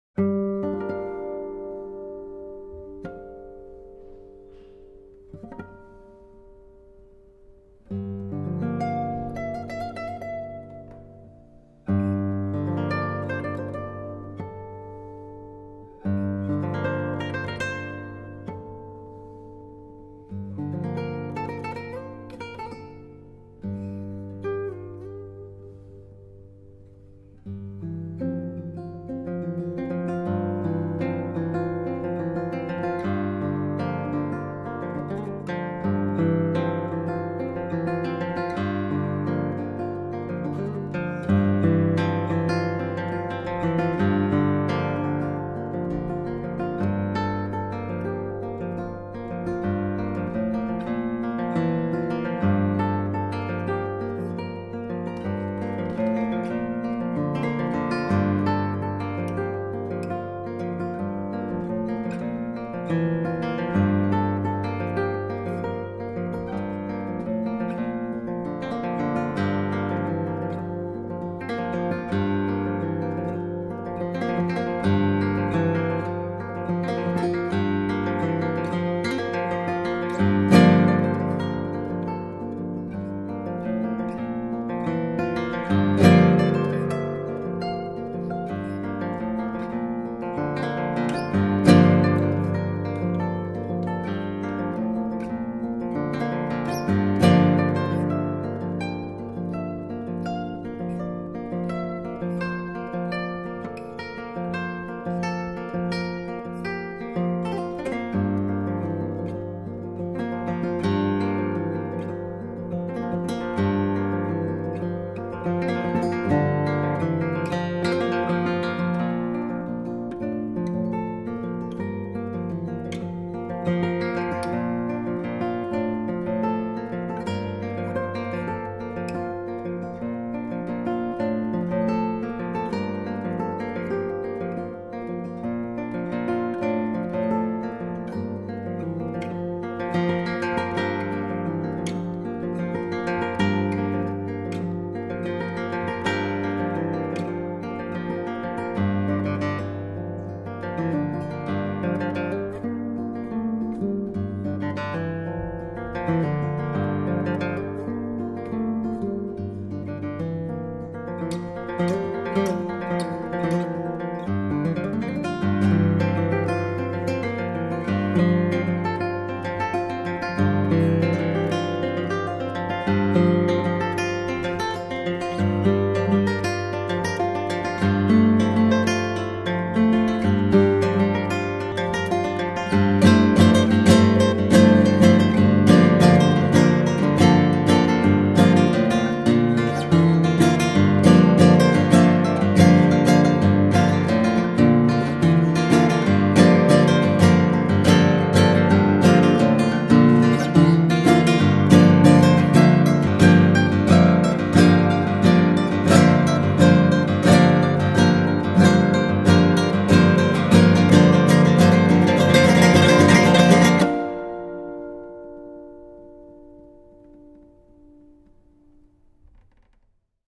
Miguel Rodriguez 1979 "Churchdoor" guitar played by Matt Bacon
flamenco-guitar.net - fg.NET - is a virtual guitar museum to promote the flamenco guitar.